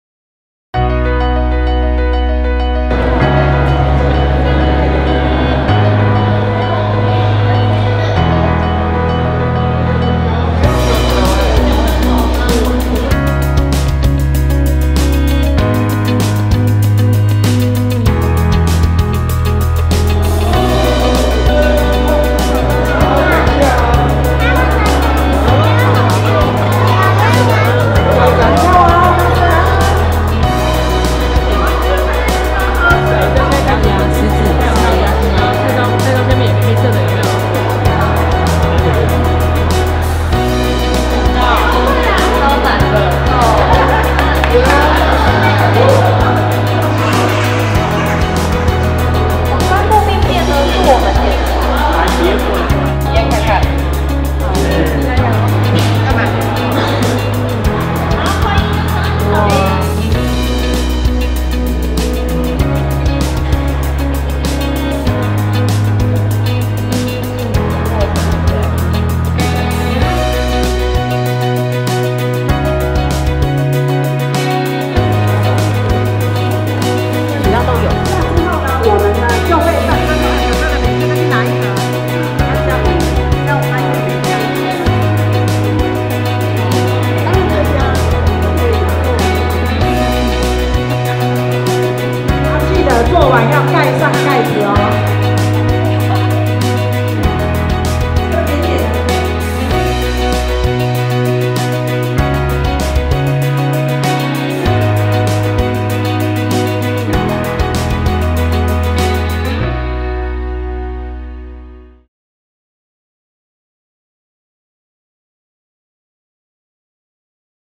108年新北市身心障礙就業成果展
由新北市勞工局主辦，透過互動展區設置、庇護工場商品展示,呈現推動身心
障礙就業服務的成果。展覽包含職業重建服務成果、成功就業案例、VR 職場體
驗、輪椅體驗，並邀請身障街頭藝人表演、DIY 工作坊。讓民眾可以更加了解
身心障礙就業的內容及故事，也提供一個交流及分享的平台。